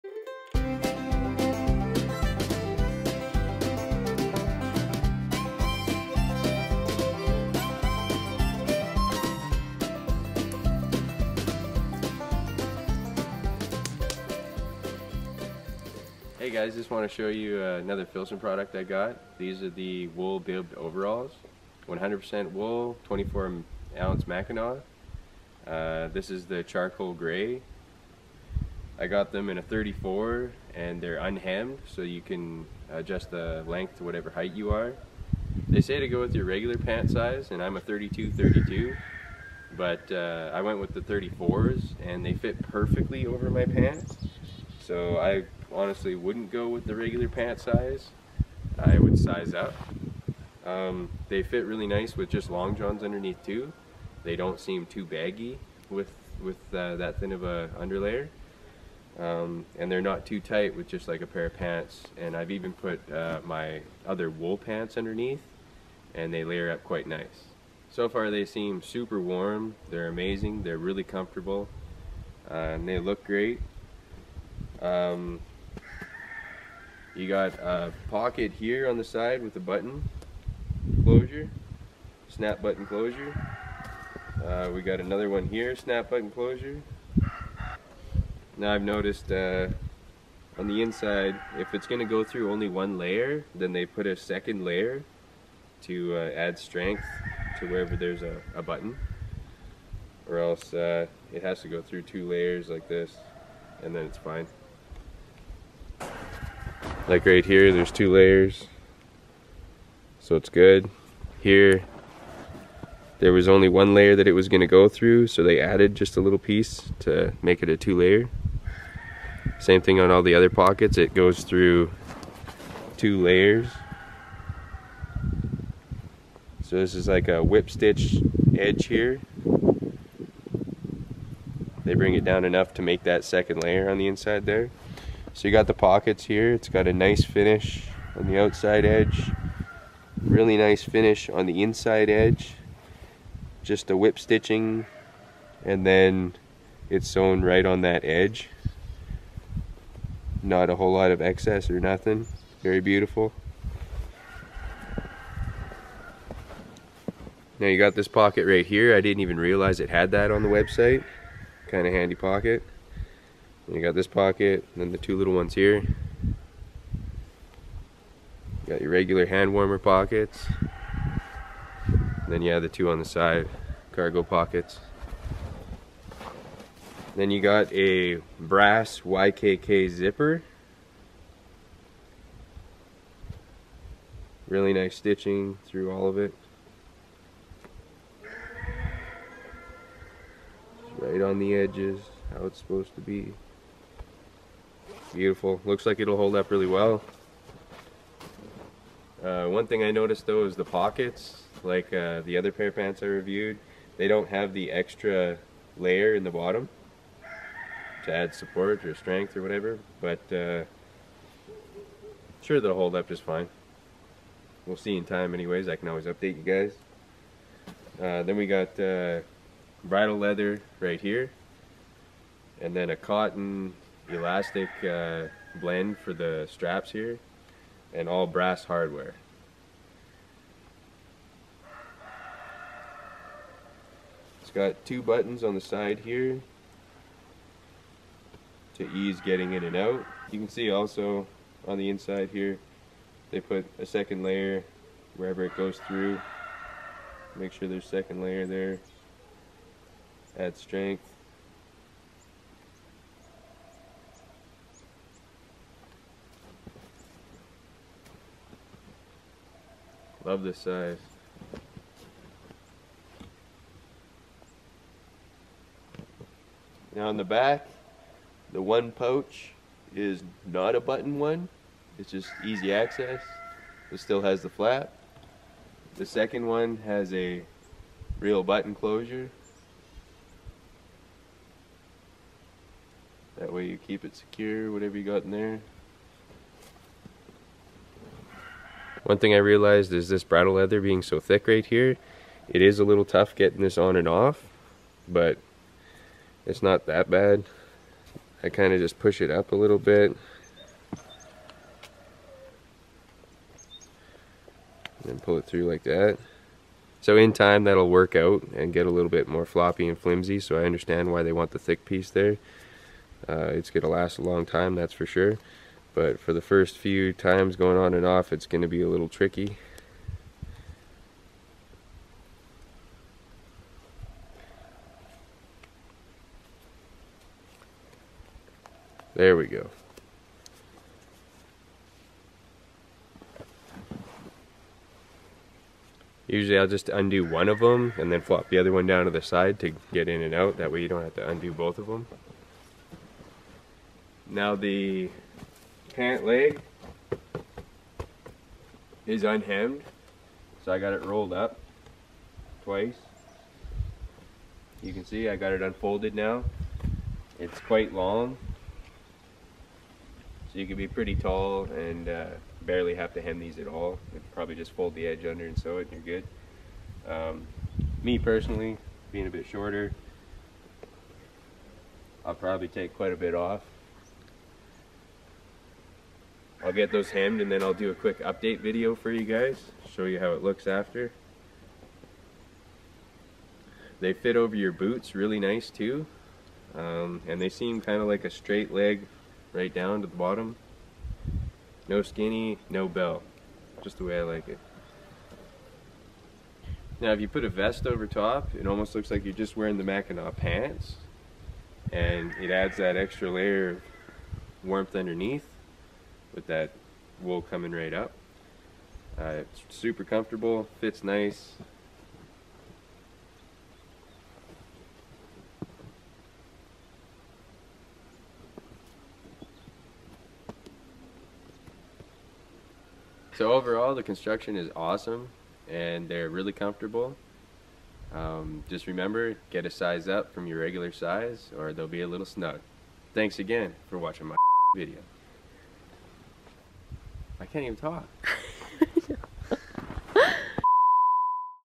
FILSON Mackinaw Wool Bibs Review
Quick filson wool bib review and try on!

In case you are unsure of what size to get, here are my measurements for reference.
Height- 5'9
Hips- 33 1/2 inches
Chest- 38 inches
Shoulders- 17 1/2 inches
Arm Length- 32 inches
Neck- 15 1/2 inches